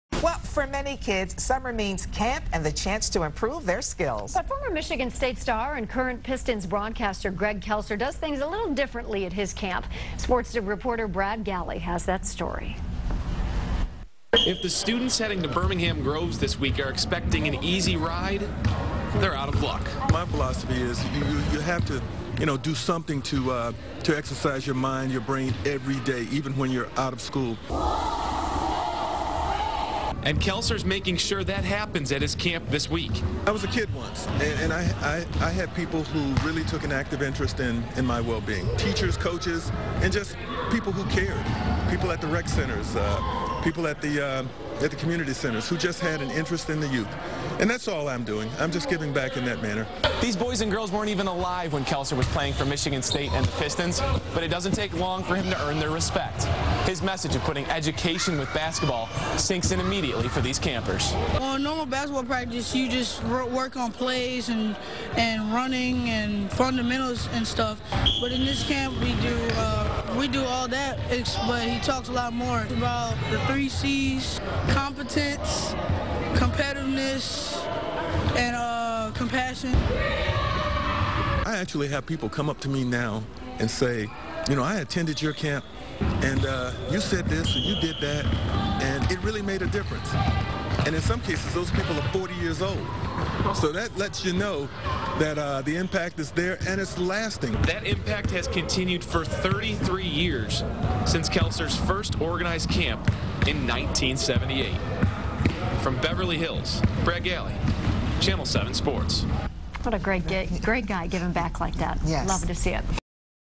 Greg Kelser's impact with basketball camps is lasting
For 33 years, Greg Kelser has worked with local boys and girls at his annual basketball camps. The impact he makes hits on more than just basketball skills. WXYZ Sports Reporter Brad Galli has the story.